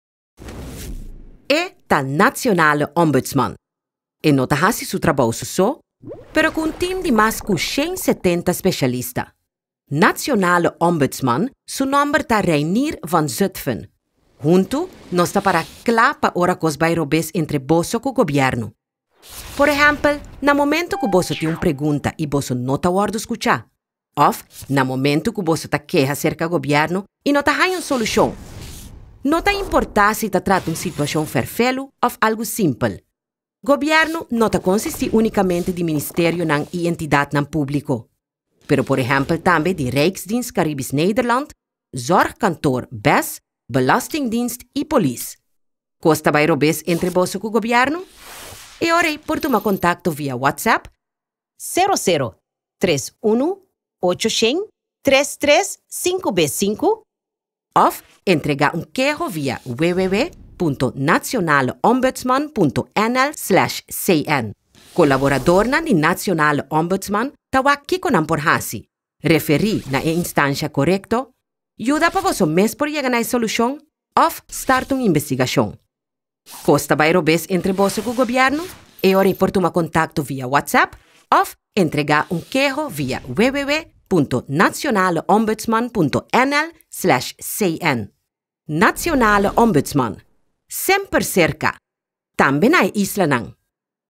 Ombudsman pa Hulanda Caribense
E ombudsman Nashonal ta semper serka di bo. Tambe pa e islanan. Bo tin un keho tokante di gobièrnu manera BES Zorgkantoor of gobièrnu lokal?